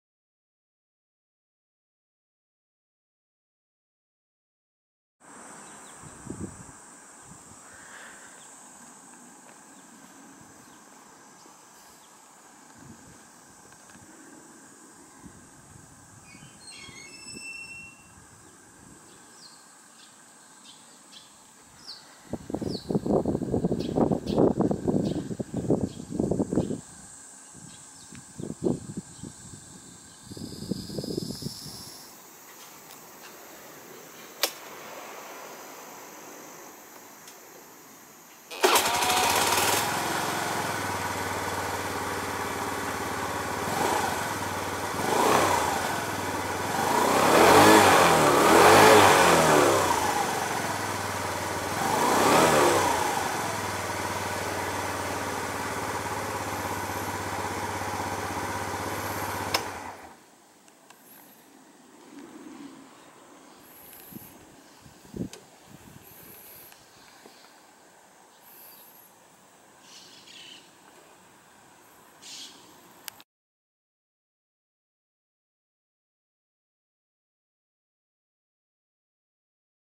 スズキ・レッツ４
乗りやすい軽量な原付です。メットインもあってお買い物にとても便利。前後タイヤ新品でエンジンオイル交換済み。しかもシート表皮も張替済みなので乗り心地や見た目もバッチリです。